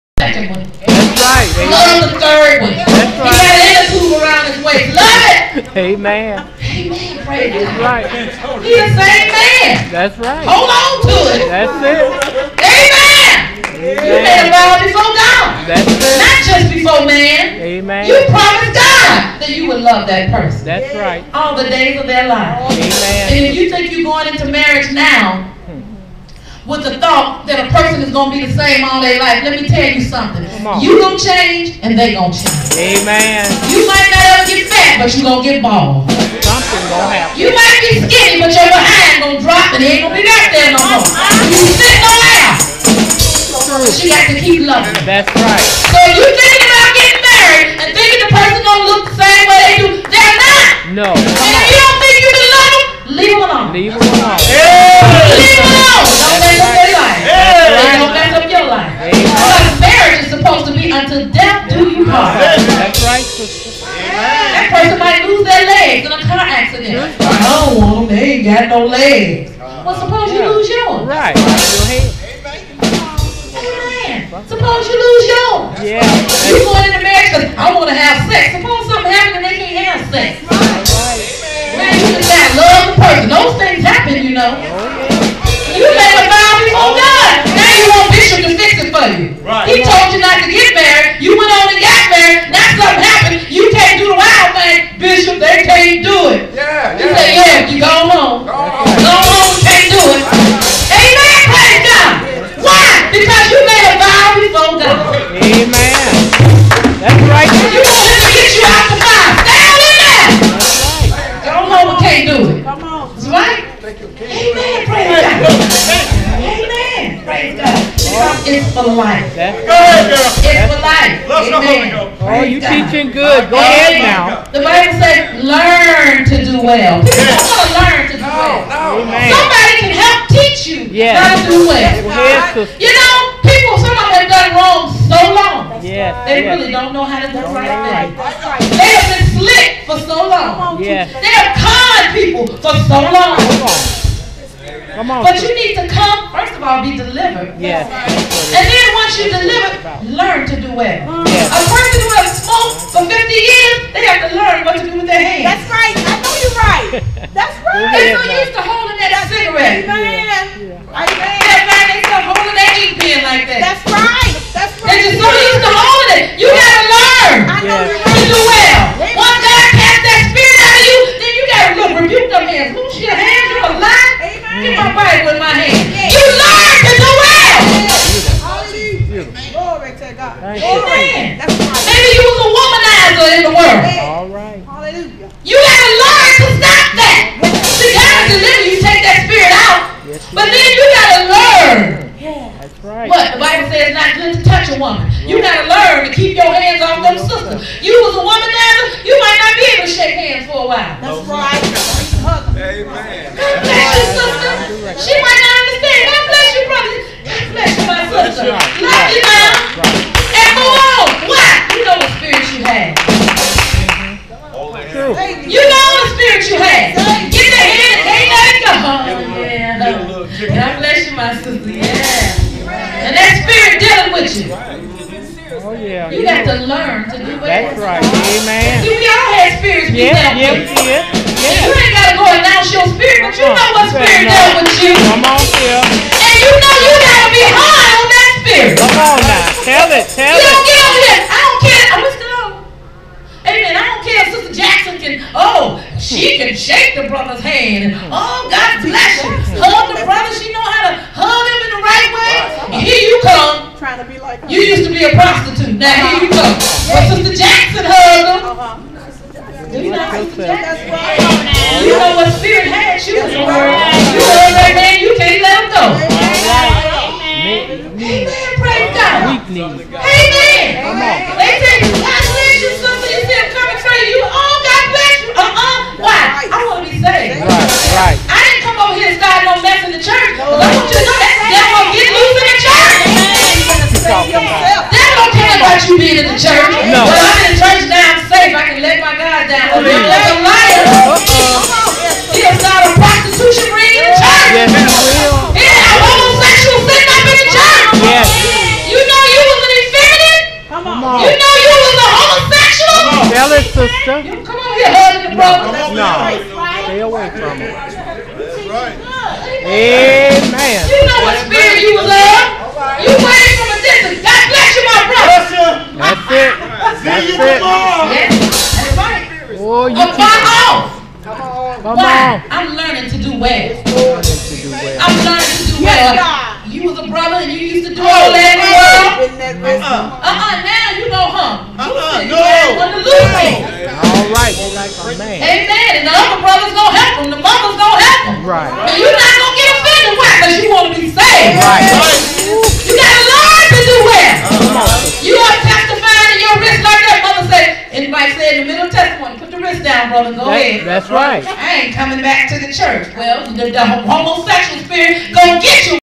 Teacher Karen Crawl -Don't Go Into A Marriage Thinking The Person Is Going To Look The Same Always
Teacher Karen Crawl - Women's Day - Don't Go Into A Marriage Thinking The Person Is Going To Look The Same Always..Marriage Is Until Death Do You Part